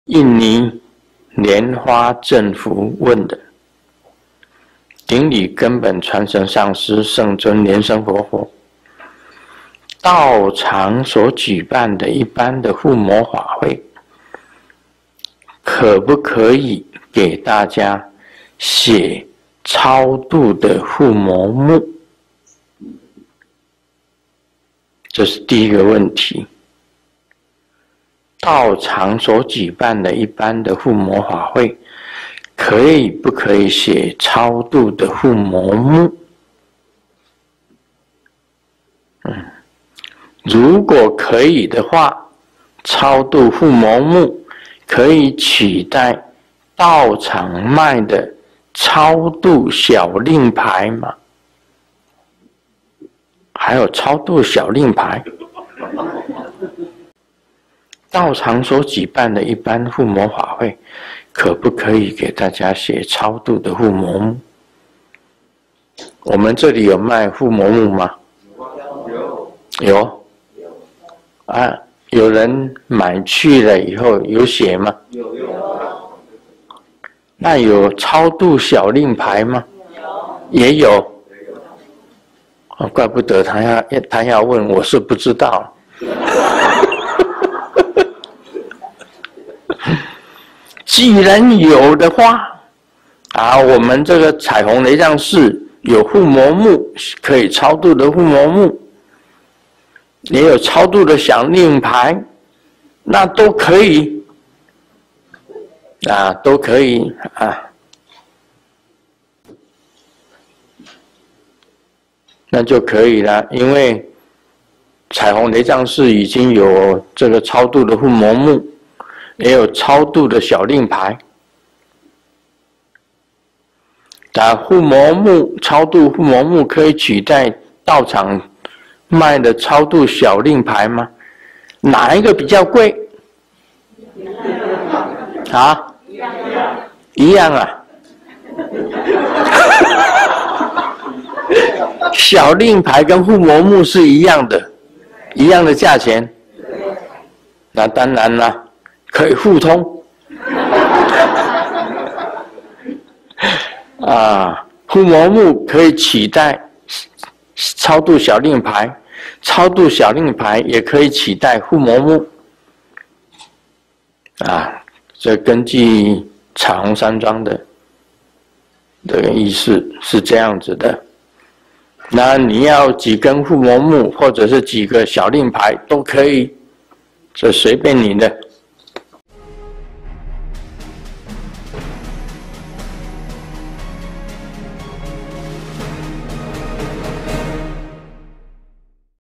20210509聖尊蓮生活佛盧勝彥解答疑難1：道場護摩法會可以用超度護摩木及超度小令牌－TBSN高清字幕版
20210509聖尊蓮生活佛盧勝彥解答疑難1：道場護摩法會可以用超度護摩木及超度小令牌－TBSN高清字幕版